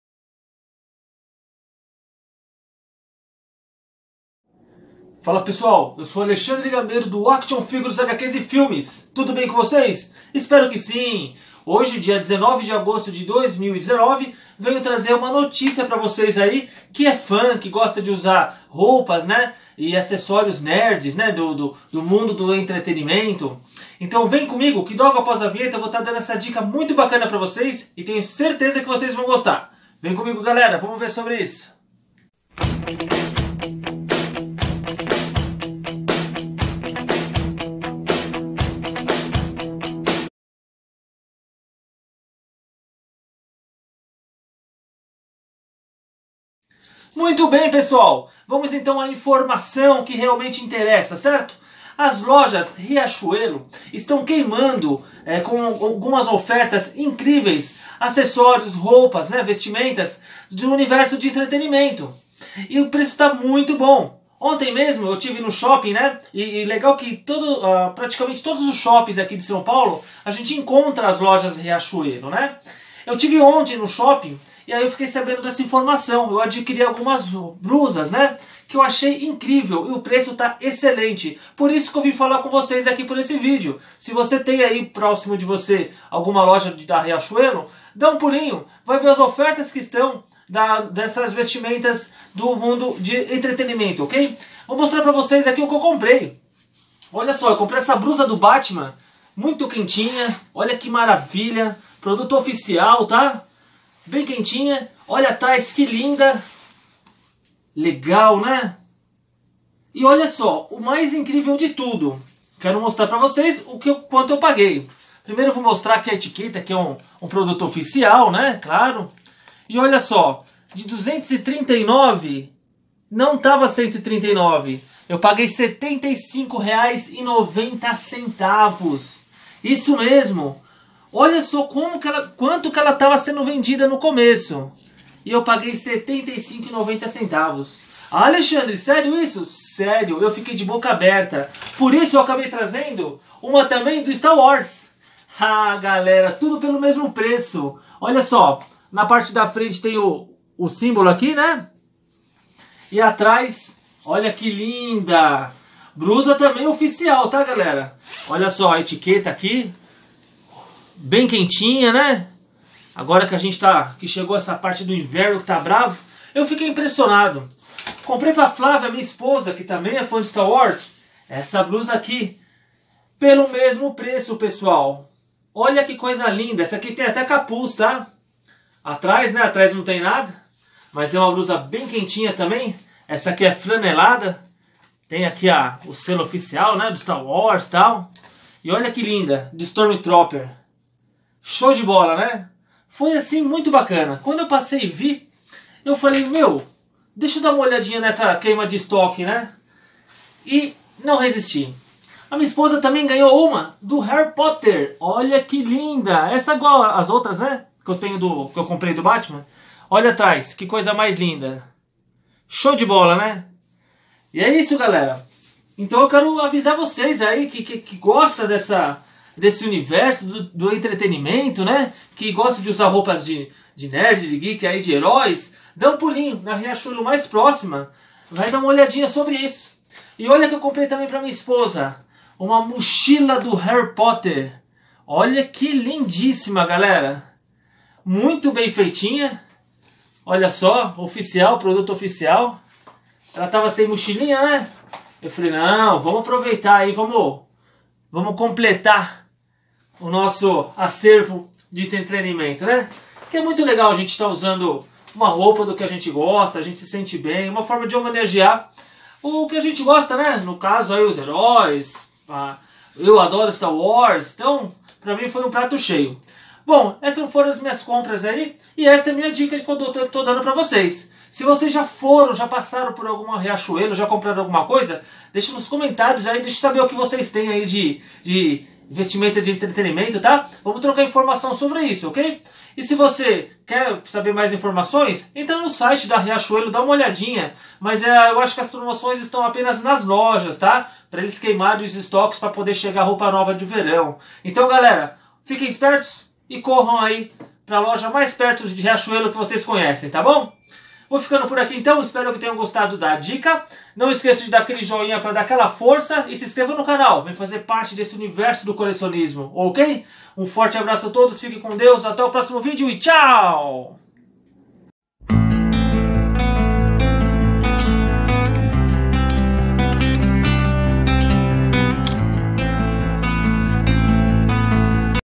Dica de "Moda Geek" nas lojas Riachuelo! (Em Promoção!) Aproveitei...
Fala pessoal!! No video de hoje venho dar uma dica pra quem quer gastar pouco com Moda Geek (Roupas e assessórios temáticos)...
Isso mesmo! As lojas Riachuelo,estão vendendo em promoção diversos artigos com temas de entretenimento... Eu aproveitei! E venho aqui para dar essa dica para vocês!! Espero que gostem!!
Curta o video e assine o canal!! Valeu!!

(alexandregameirocaparroz)

Um forte abraço a todos! Fiquem com Deus e até o próximo video!!

VALEU!!